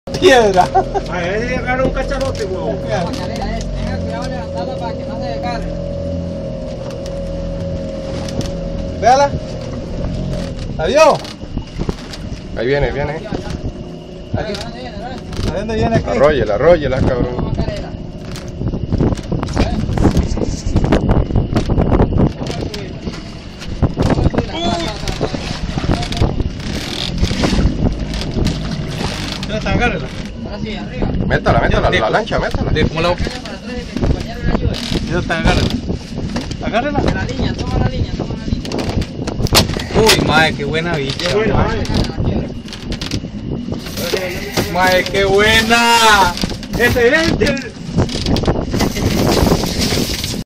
pesca de macarela
Macarela pequeña , sacada el 02,02, 2013 , cerca de drake ,zona sur , Costa Rica . Señuelo Yozuri.f